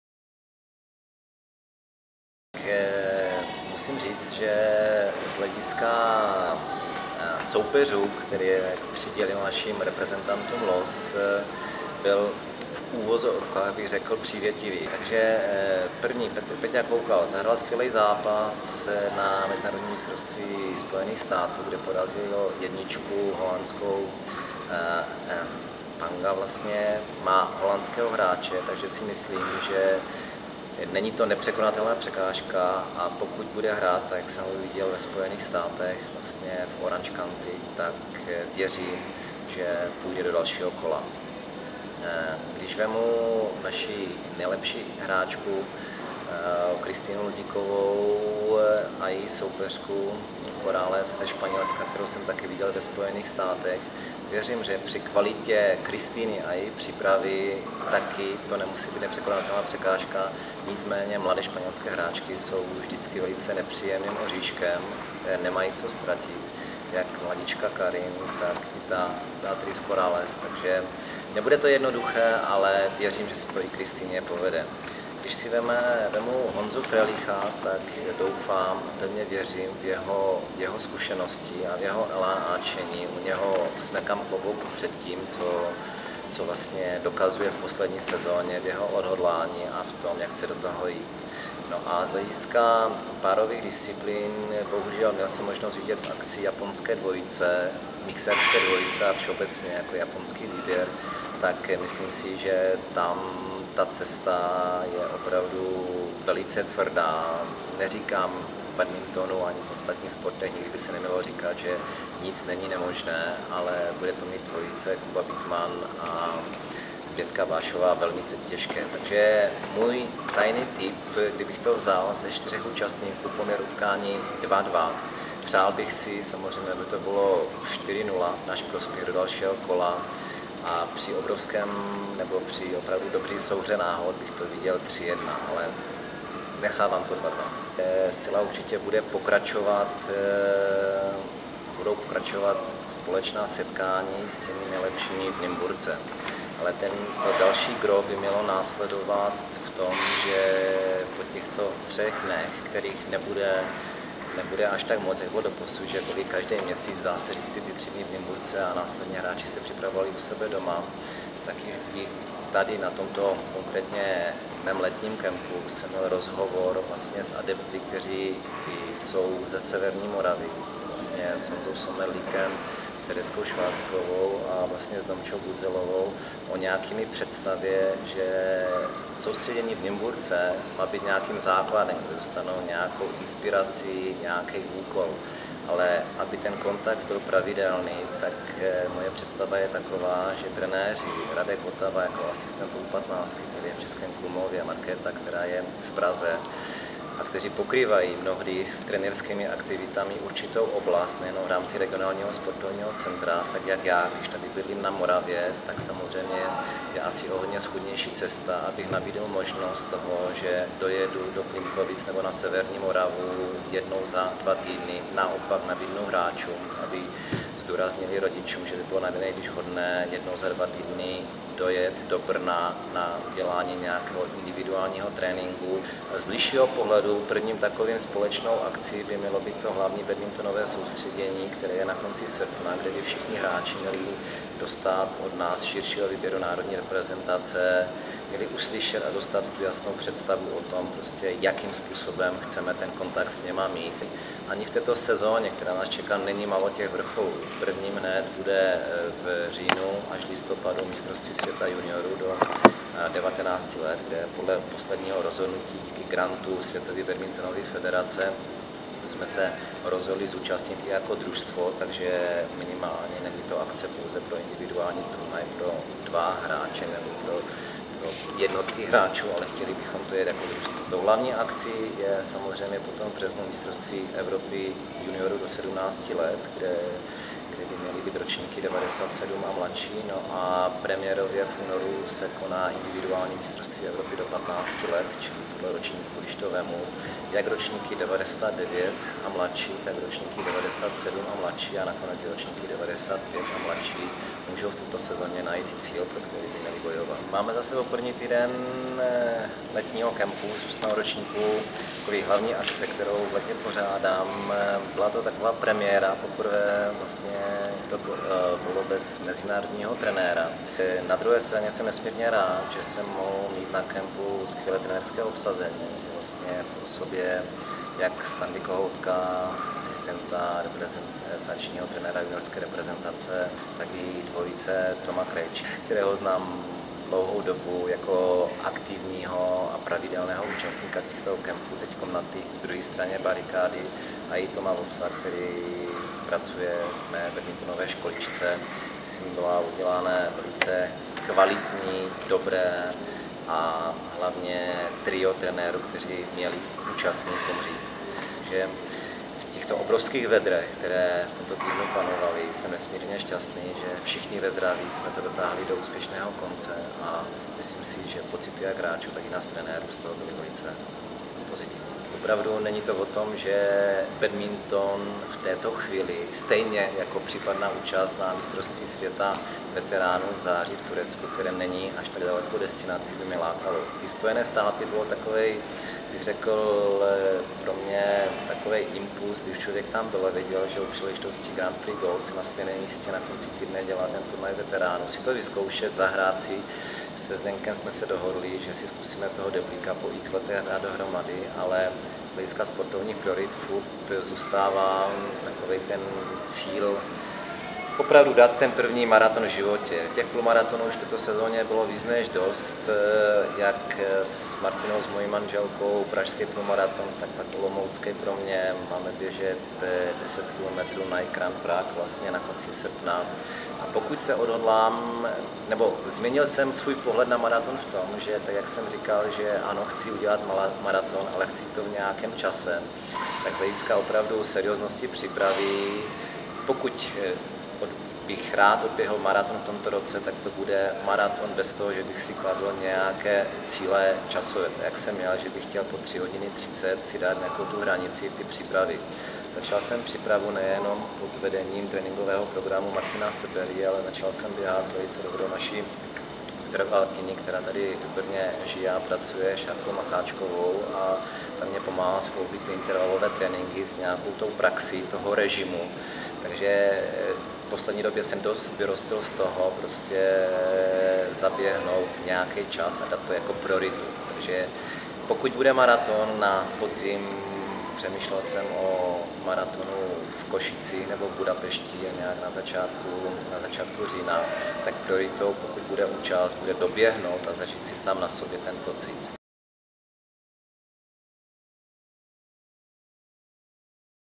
Tomasz Mendrek: rozhovor o MS dospělých i reprezentaci juniorů
Předseda trenérko-metodické komise a trenér národní reprezentace Tomasz Mendrek odpovídal na otázky reportéra BadmintonWebu. V rozhovoru uslyšíte jeho názor na rozlosování MS jednotlivců, změny v přípravě reprezentace juniorů a vrcholy sezóny pro juniorské národní družstvo, i hodnocení 8. ročníku letního soustředění Badminton School. A dojde i na osobní sportovní cíle v badmintonu i běhání.